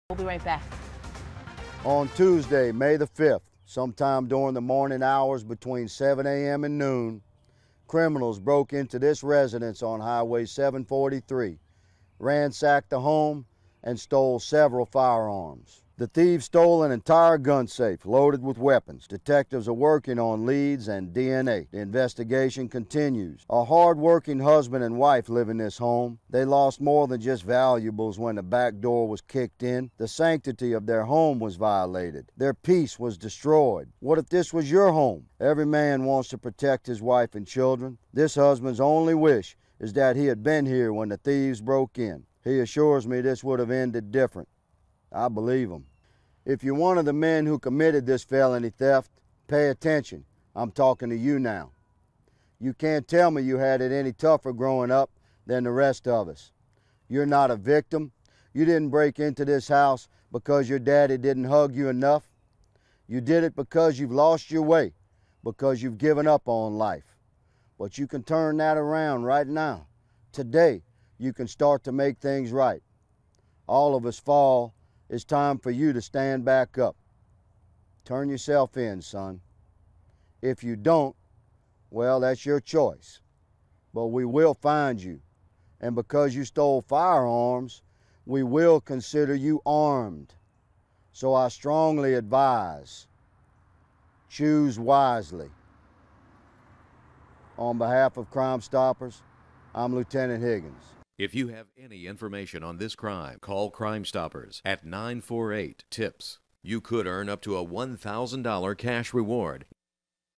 Crimestoppers for May 13, 2015- Home Burglary
&nbsp;St. Landry Crimestoppers for May 13, 2015 with Lt. Clay Higgins- Home Burglary